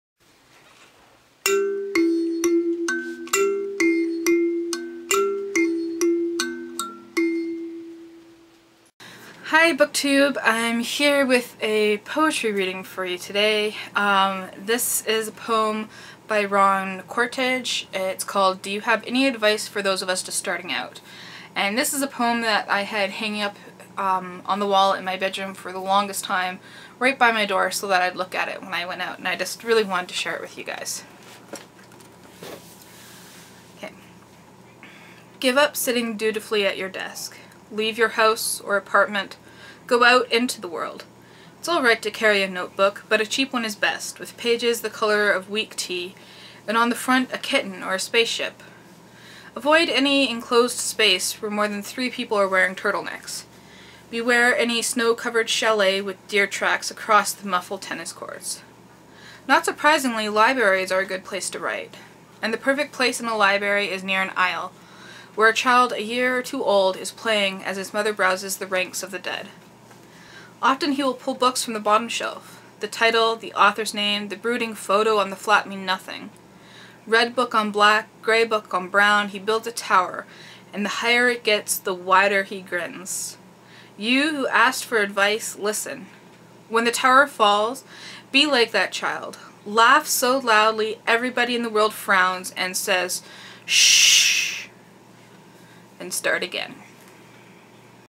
VEDA # 18 | Poetry: Do You Have Any Advice For Those Of Us Just Starting Out
Do You Have Any Advice For Those of Us Just Starting Out by Ron Koerge

Let's talk about books!